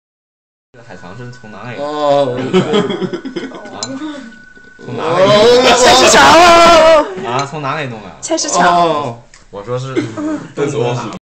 Scary sea intestines
Here is some sea intestines, tastes like balloons